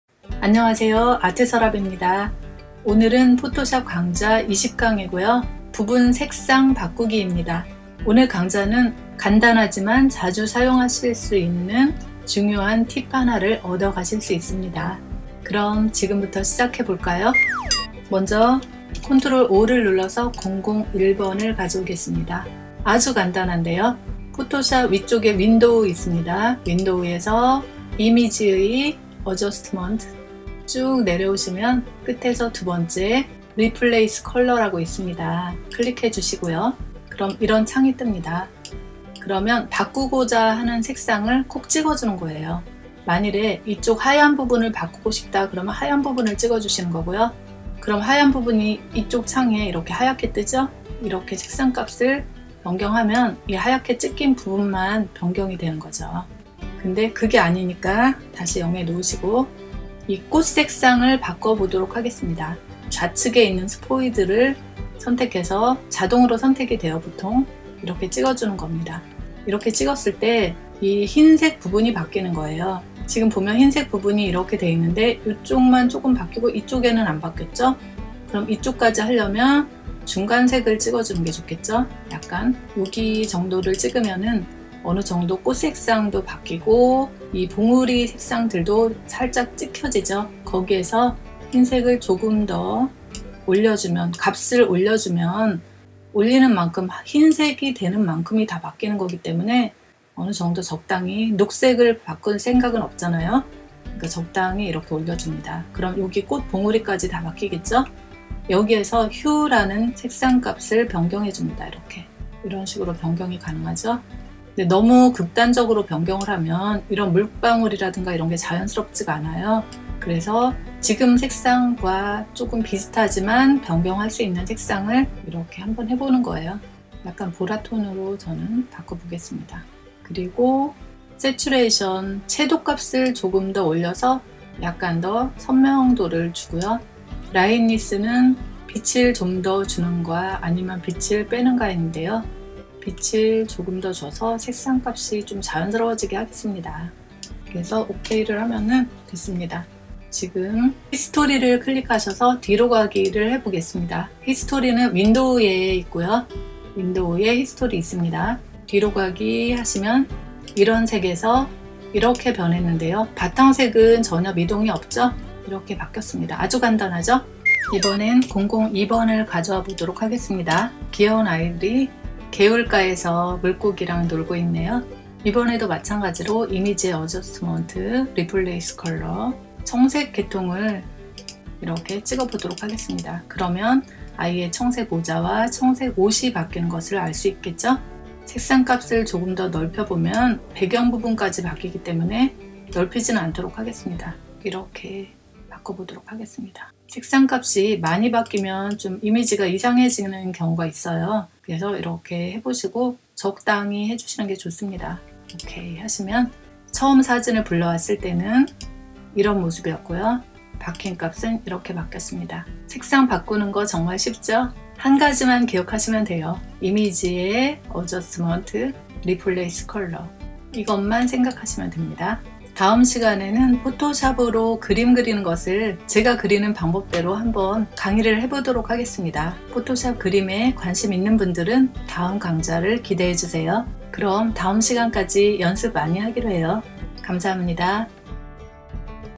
[포토샵강좌 20] 👍 사진색상 바꾸기 아주 쉬워요, 사진 색상보정으로 부분색상바꾸기, 색상변경
사진색상변경 하는 간단한 방법 색상바꾸기 포토색감보정. 얼굴색이나 일부 옷 색상이나 하늘 등 다양하게 일부분색상을 바꿀 수 있어요~
아이들사진. Pixabay로부터 입수된 Hai Nguyen Tien님의 이미지 입니다.